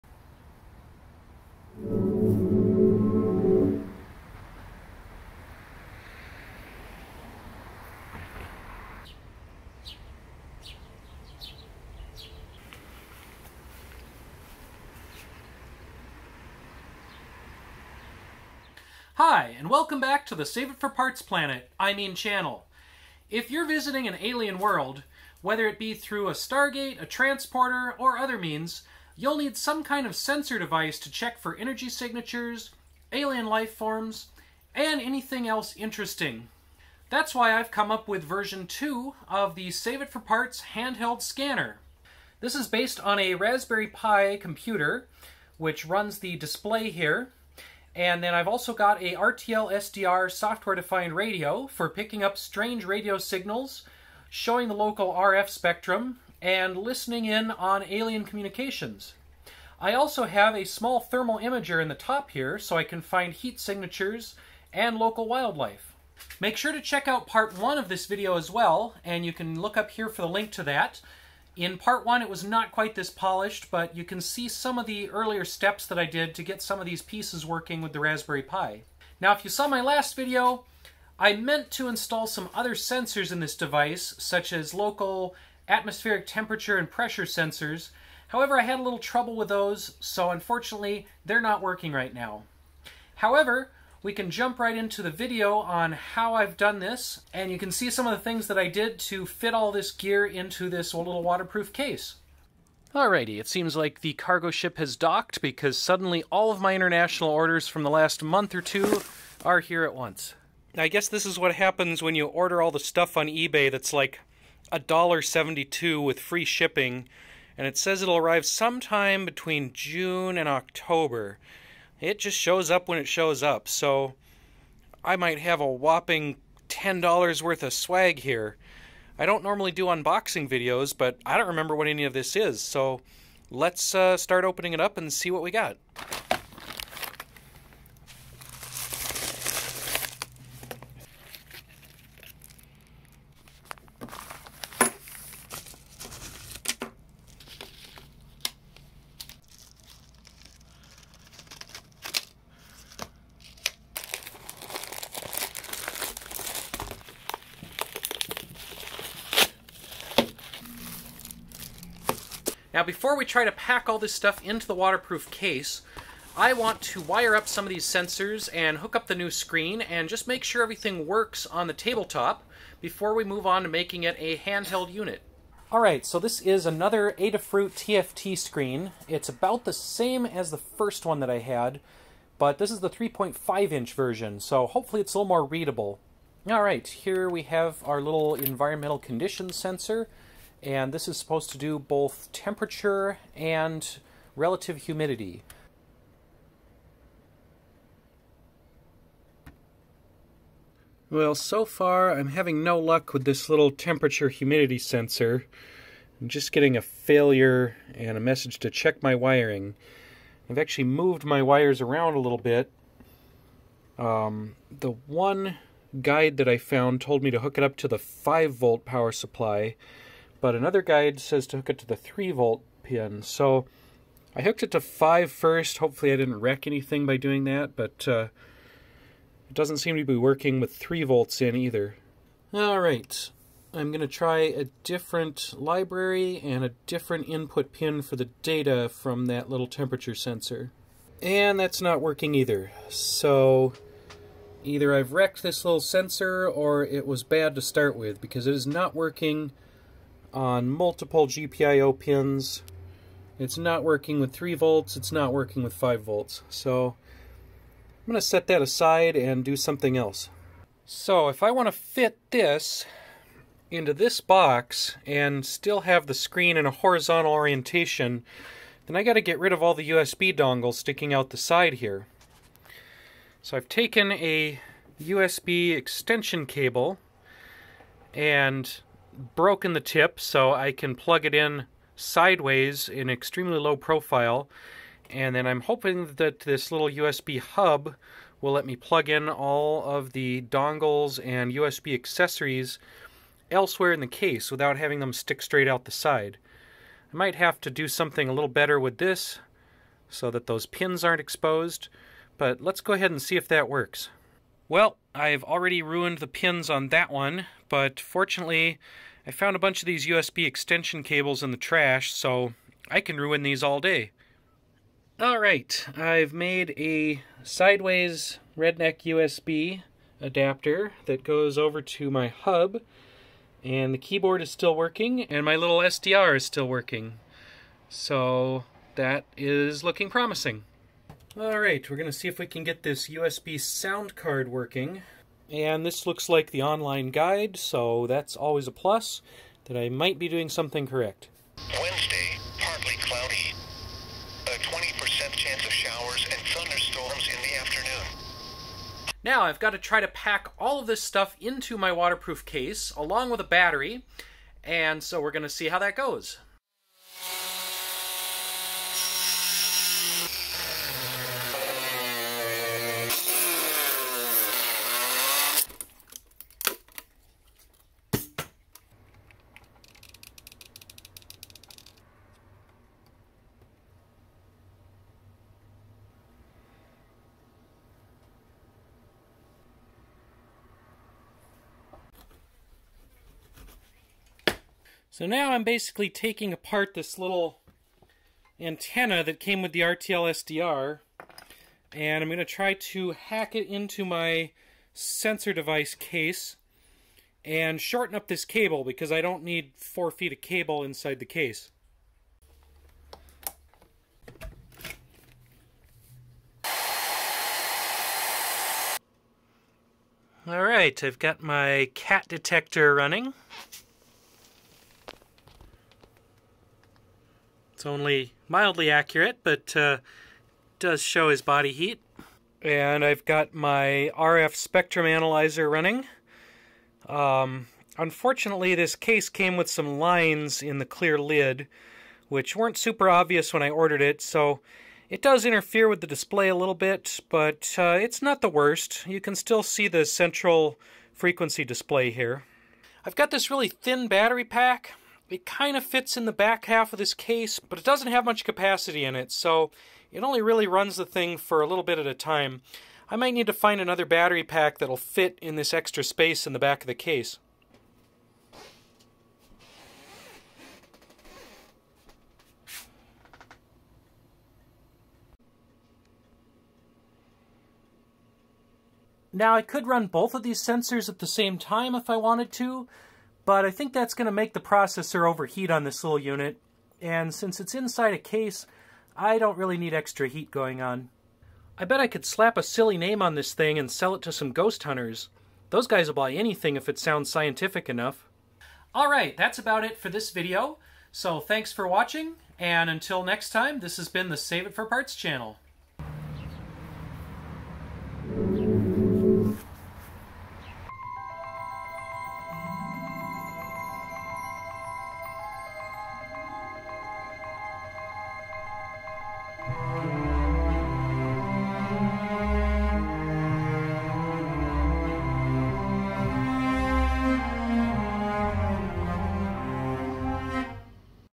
Handheld Scanning Device with Raspberry Pi - Part 2
Part 2 of my handheld scanner / sensor thingy made from a Raspberry Pi. It includes a software defined radio and a thermal imager (I'm still working on some other sensors!)

Much thanks to @eatstoomuchjam for the Raspberry Pi!